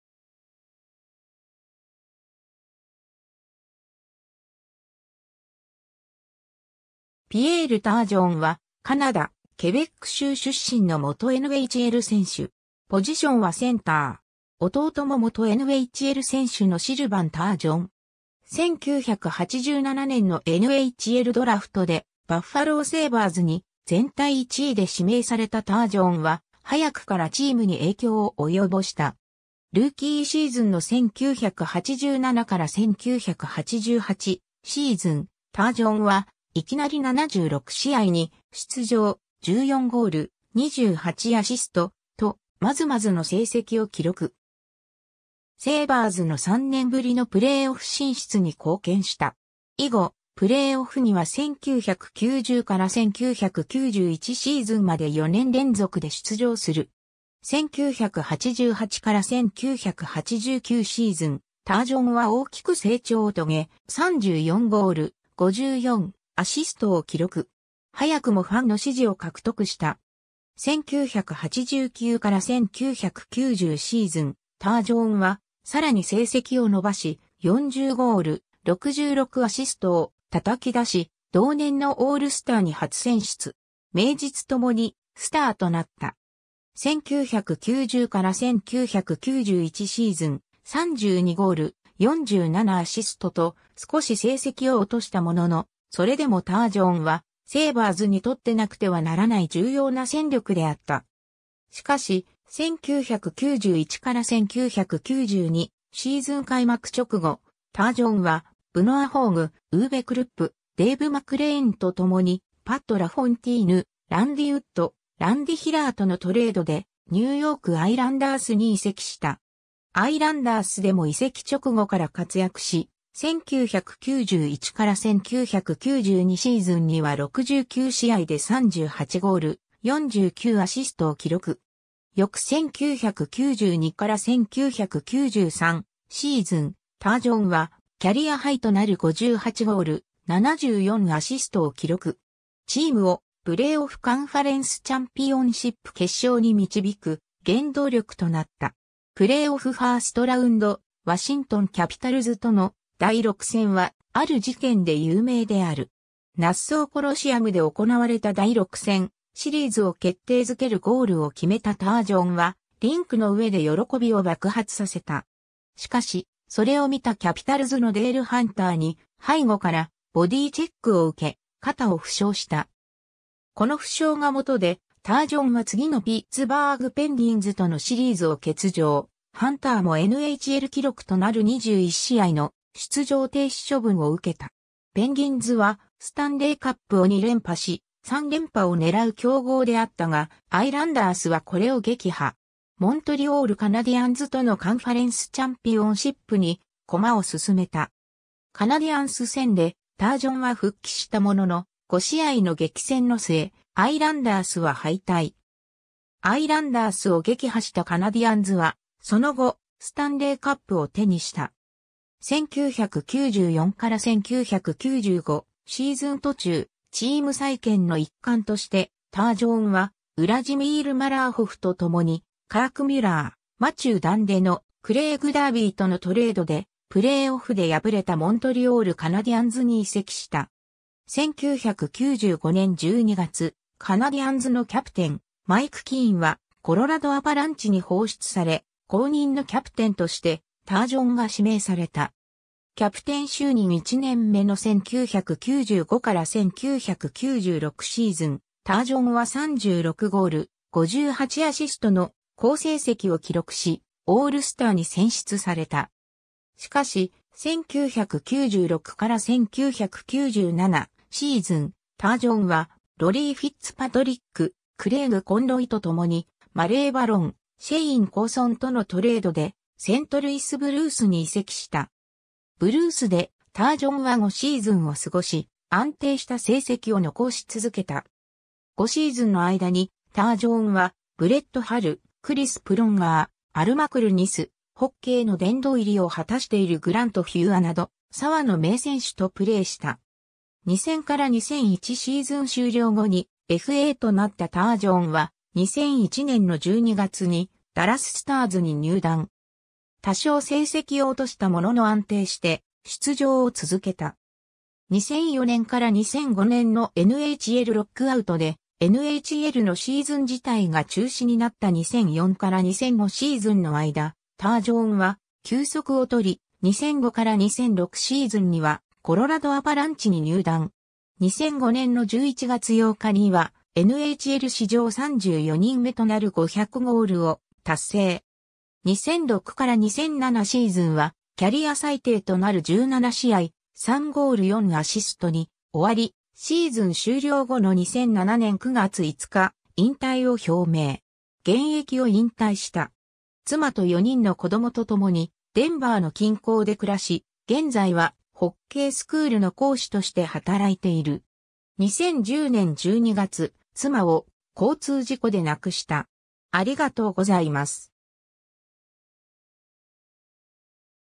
ピエール・タージョン
バッファロー・セイバーズの選手
ニューヨーク・アイランダースの選手
モントリオール・カナディアンズの選手
セントルイス・ブルースの選手
ダラス・スターズの選手
コロラド・アバランチの選手
ピエール・タージョン（Pierre Turgeon, 1969年12月28日 - ）は、カナダ、ケベック州出身の元NHL選手。
弟も元NHL選手のシルヴァン・タージョン。
 1987年のNHLドラフトで、バッファロー・セイバーズに全体1位で指名されたタージョンは、早くからチームに影響を及ぼした。
 ルーキーシーズンの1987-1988シーズン、タージョンはいきなり76試合に出場、14ゴール、28アシスト、とまずまずの成績を記録。
セイバーズの3年ぶりのプレーオフ進出に貢献した。
以後、プレーオフには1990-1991シーズンまで4年連続で出場する。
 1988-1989シーズン、タージョンは大きく成長を遂げ、34ゴール、54アシストを記録。
早くもファンの支持を獲得した。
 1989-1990シーズン、タージョンは更に成績を伸ばし、40ゴール、66アシストを叩き出し、同年のオールスターに初選出。
名実ともにスターとなった。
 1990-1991シーズン、32ゴール、47アシストと、少し成績を落としたものの、それでもタージョンはセイバーズにとってなくてはならない重要な戦力であった。
 しかし、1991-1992シーズン開幕直後、タージョンは、ブノア・ホーグ、ウーヴェ・クルップ、デイブ・マクレーンとともに、パット・ラフォンティーヌ、ランディ・ウッド、ランディ・ヒラーとのトレードで、ニューヨーク・アイランダースに移籍した。
 アイランダースでも移籍直後から活躍し、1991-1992シーズンには69試合で38ゴール、49アシストを記録。
 翌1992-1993シーズン、タージョンはキャリアハイとなる58ゴール、74アシストを記録。
チームをプレーオフカンファレンスチャンピオンシップ決勝に導く原動力となった。
 プレーオフファーストラウンド、ワシントン・キャピタルズとの第6戦は、ある事件で有名である。
 ナッソー・コロシアムで行われた第6戦、シリーズを決定づけるゴールを決めたタージョンは、リンクの上で喜びを爆発させた。
しかし、それを見たキャピタルズのデール・ハンターに背後からボディーチェック（体当たりのこと。原則としてパックを持っていない選手にはしてはいけない）を受け、肩を負傷した。
この負傷がもとでタージョンは次のピッツバーグ・ペンギンズとのシリーズを欠場、ハンターもNHL記録となる21試合の出場停止処分を受けた。
 ペンギンズはスタンレー・カップを2連覇し、3連覇を狙う強豪であったが、アイランダースはこれを撃破。
モントリオール・カナディアンズとのカンファレンス・チャンピオンシップに駒を進めた。
 カナディアンス戦でタージョンは復帰したものの、5試合の激戦(2試合は延長線までもつれ込んだ)の末、アイランダースは敗退。
アイランダースを撃破したカナディアンズはその後、スタンレー・カップを手にした。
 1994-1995シーズン途中、チーム再建の一環として、タージョンはウラジミール・マラーホフとともに、カーク・ミュラー、マチュー・ダンデノ、クレイグ・ダービーとのトレードで、プレーオフで敗れたモントリオール・カナディアンズに移籍した。
 1995年12月、カナディアンズのキャプテン、マイク・キーンはコロラド・アバランチに放出され、後任のキャプテンとしてタージョンが指名された。
 キャプテン就任1年目の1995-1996シーズン、タージョンは36ゴール、58アシストの好成績を記録し、オールスターに選出された。
 しかし、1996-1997シーズン、タージョンはロリー・フィッツパトリック、クレイグ・コンロイとともに、マレー・バロン、シェイン・コーソンとのトレードで、セントルイス・ブルースに移籍した。
 ブルースでタージョンは5シーズンを過ごし、安定した成績を残し続けた。
5シーズンの間にタージョンは、ブレット・ハル、クリス・プロンガー、アル・マクルニス、ホッケーの殿堂入りを果たしているグラント・フューアなど、多の名選手とプレーした。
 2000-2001シーズン終了後にFAとなったタージョンは、2001年の12月にダラス・スターズに入団。
多...